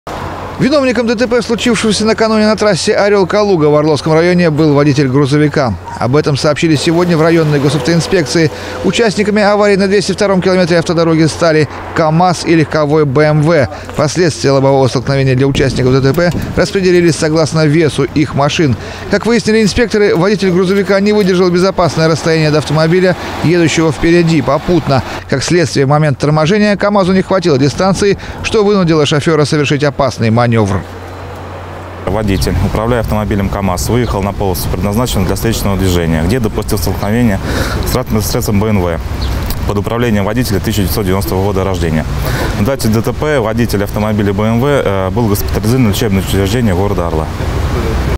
Назван виновник вчерашнего ДТП на выезде из Орла
Участниками аварии на 202-м километре автодороги стали "Камаз" и легковая "БМВ". Последствия лобового столкновения для участников ДТП распределились согласно весу их машин. Как выяснили инспекторы, водитель грузовика не выдержал безопасное расстояние до автомобиля, едущего впереди попутно. Как следствие, в момент торможения "Камазу" не хватило дистанции, что вынудило шофёра совершить опасный манёвр.